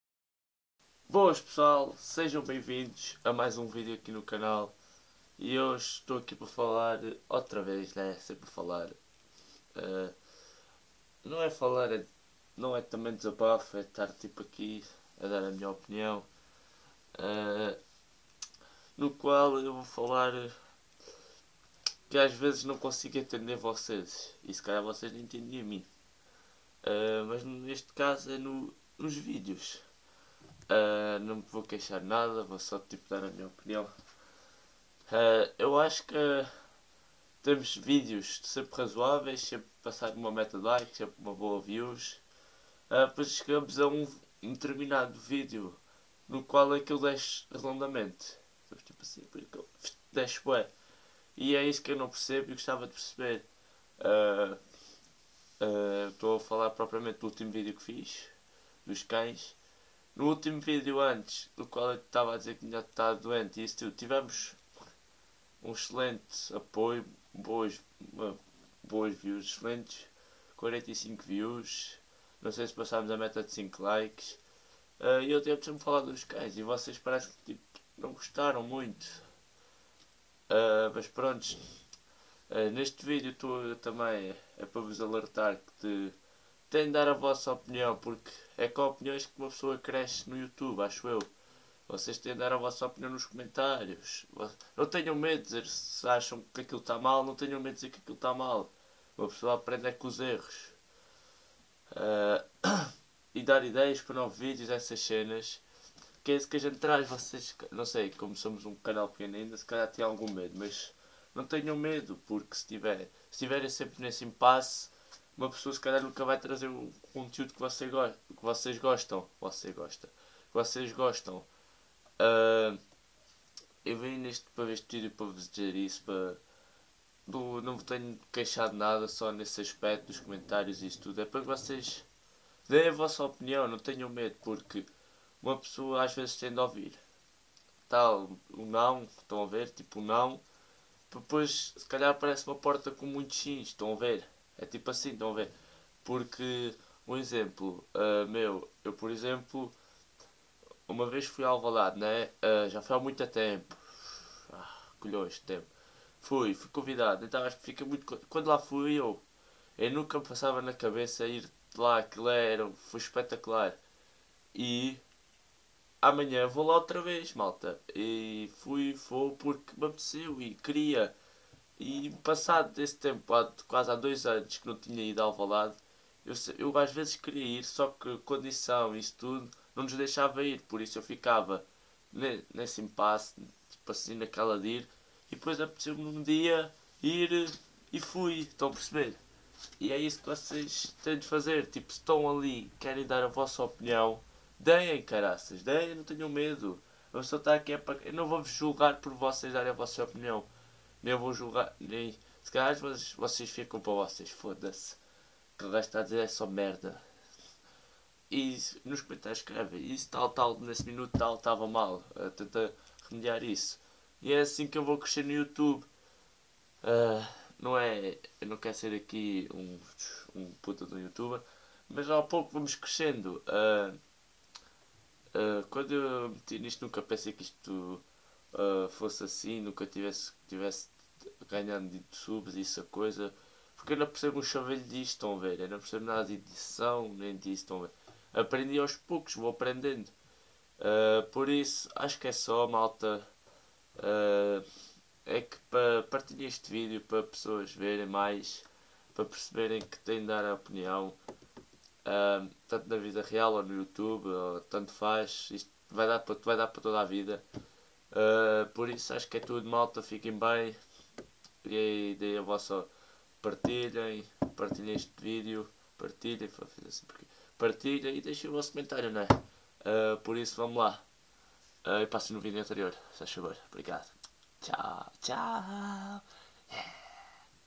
Digam a vossa opinião...
Manos mais um video aqui no canal e desta vez foi um video no qual tive a falar sobre vocês darem a vossa opinião nos videos.
Espero que partilhem o video ao máximo e dêem ideias para próximos videos.
E fuiiiiiiiiiiiiiiiiiiii!!!